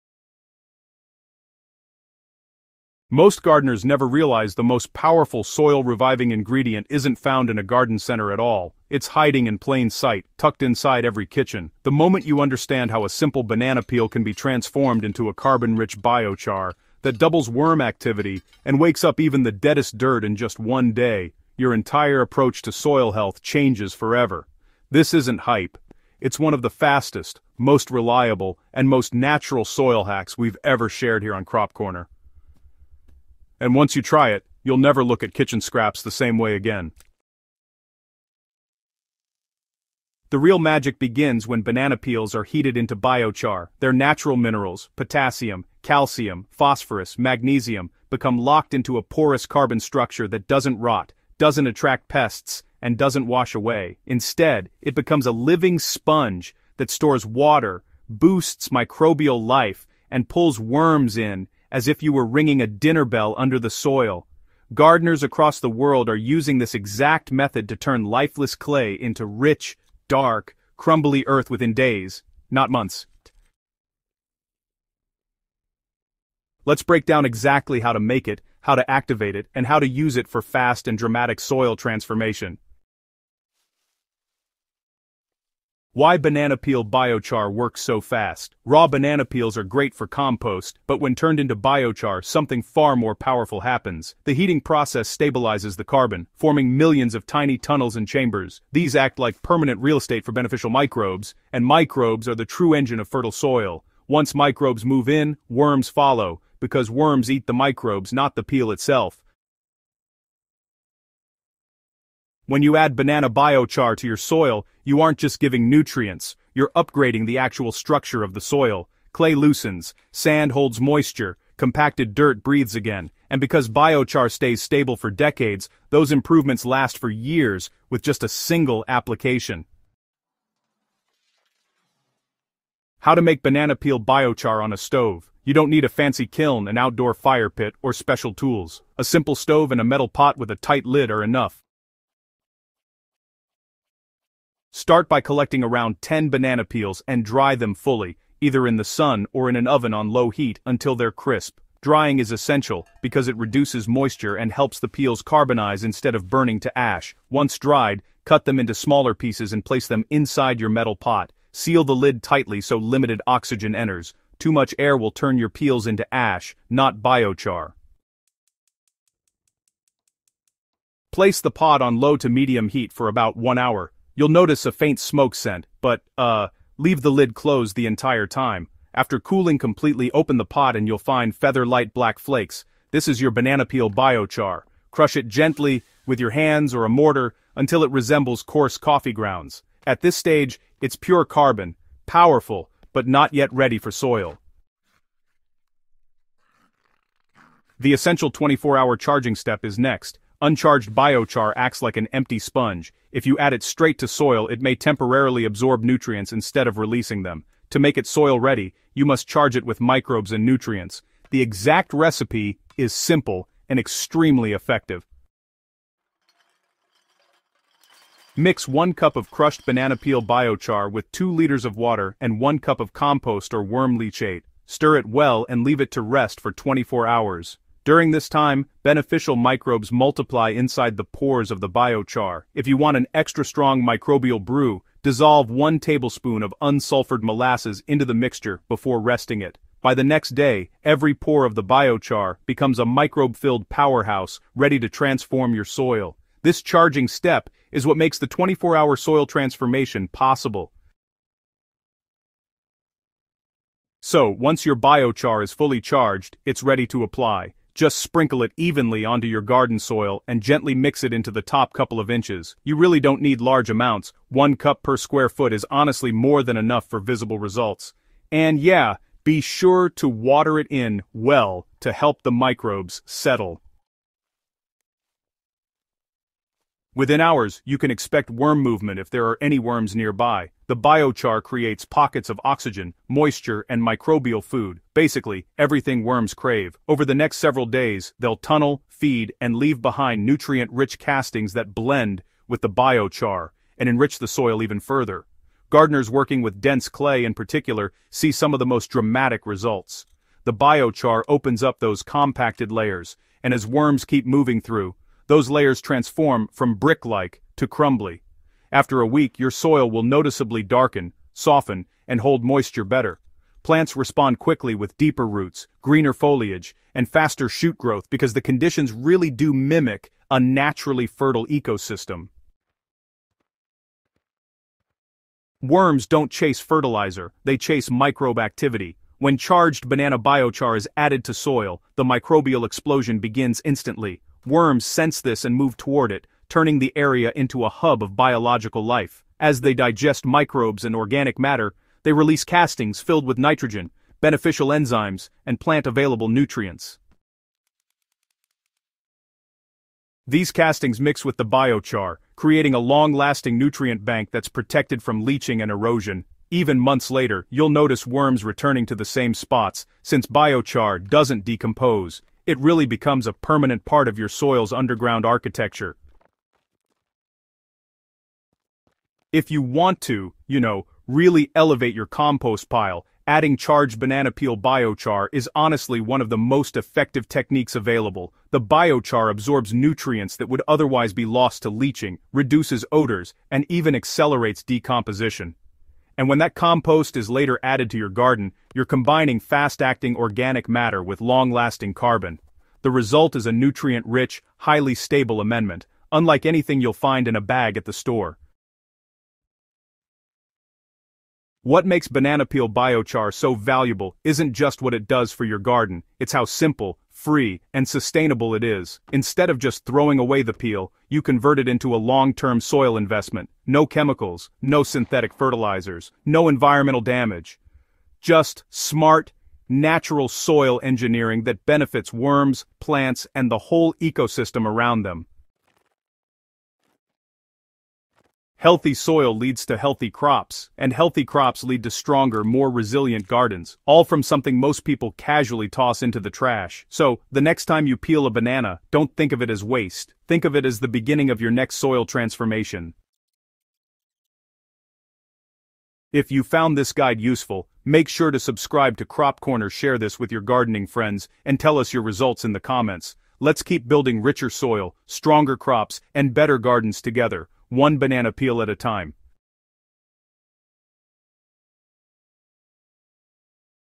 Banana Peel Biochar: The 24-Hour Soil Hack That Doubles Worms and Revives Dead Dirt Fast!
This video reveals how banana peel biochar—a simple, homemade carbon-rich soil booster—can transform struggling soil faster than compost, fertilizers, or store-bought amendments. With just a stove, a pot, and a handful of banana peels, you can create a microbe-loaded, worm-attracting, soil-reviving powerhouse that works instantly.

You’ll learn:
• How to turn ordinary banana peels into pure black biochar
• The exact formula to charge it with microbes for maximum results
• Why worms swarm to it within hours
• How it softens clay, boosts root growth, and revives lifeless dirt
• The secret reason biochar stays active in soil for decades
• How to use it to supercharge your compost pile

Gardeners who use this method report double the worm activity, noticeably softer soil, and deeper root growth in just a week. This isn’t theory—this is real, practical soil science you can make at home today.

If you’re tired of battling stubborn soil, weak plant growth, or dry lifeless garden beds, this is the game-changer you’ve been waiting for.

💛 Turn kitchen waste into the most powerful soil amendment you’ll ever make.
🌱 Revive dead soil. Boost worms. Grow stronger plants.
⚡ All in just 24 hours.

Together, let’s bring soil back to life—one banana peel at a time.